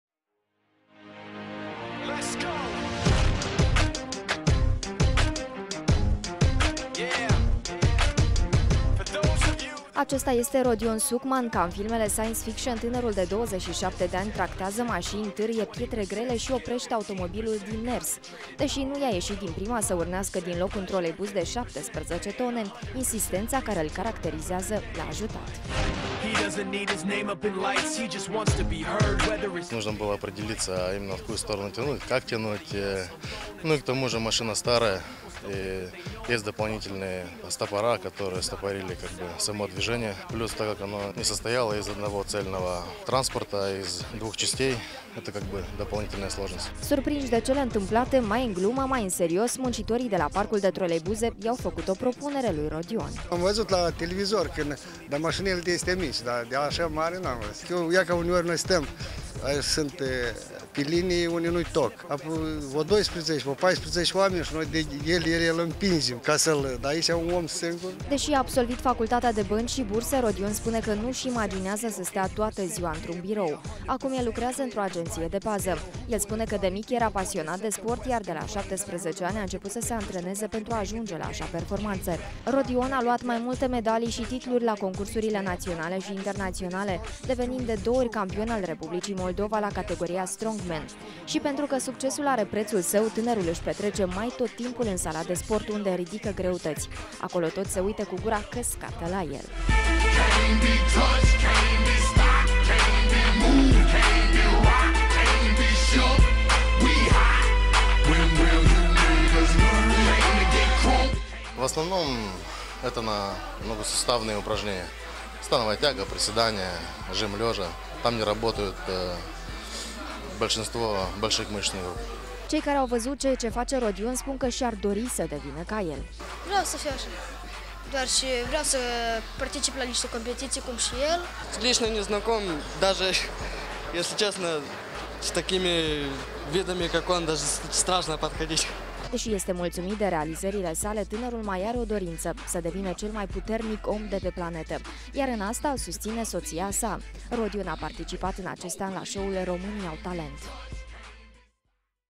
Cel mai puternic om din Moldova indoaie fier si tracteaza troleibuze
Tracteaza masini de cateva tone, indoaie obiecte din fier si roteste lucruri grele. Nu vorbim despre un erou din desene animate ci despre moldoveanul - Rodion Sucman. Barbatul se poate considera pe drept cel mai puternic om din tara, insa nu se multumeste cu asta. Visul lui este sa devina cel mai puternic om de pe planeta.